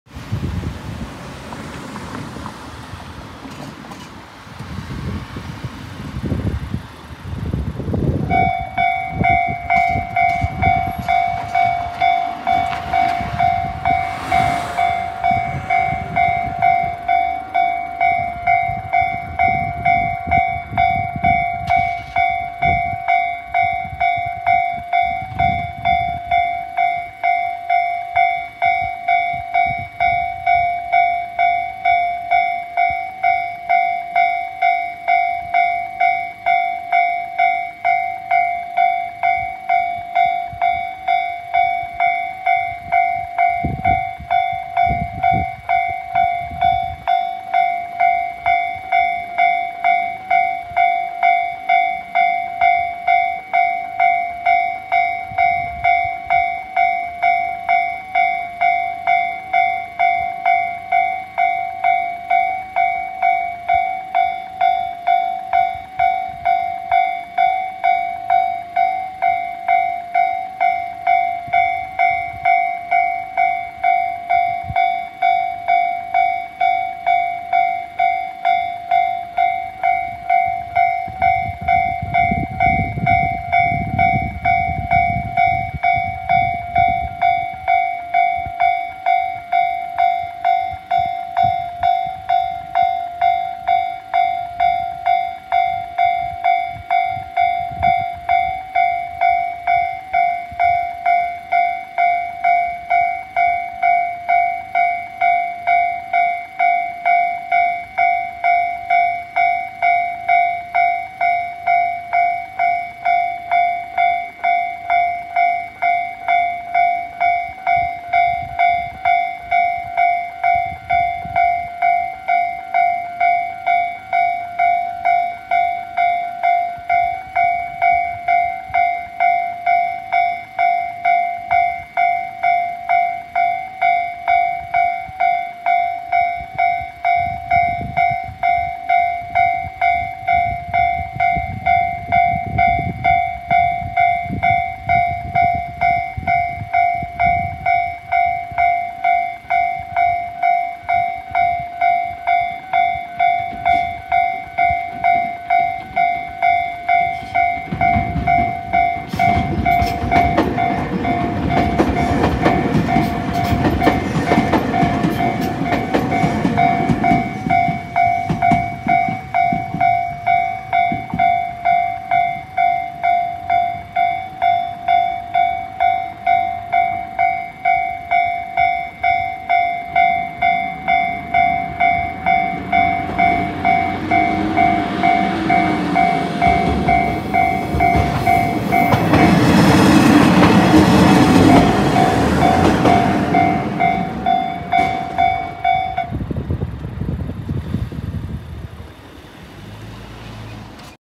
【2023年1月1日撮影】東武桐生線／わたらせ渓谷鐵道 新桐生・下新田ー相老 踏切
音→東武：東上タイプ
　　わ鐡：通常音
東武　新桐生←踏切→相老
わ鐡　下新田←踏切→相老
桐　第80号踏切
前橋街道踏切